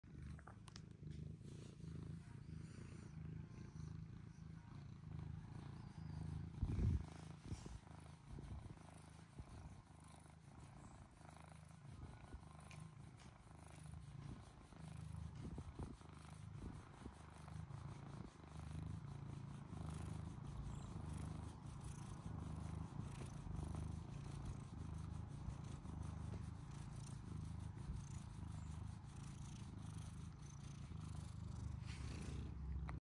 Kitten Loves Chin Scratches
Kitten Loves to Get His Chin Scratched

Cat In The Video : Charlie (British Shorthair Male Kitten)

Cat purring while getting some chin scratches

Cat enjoying chin scratches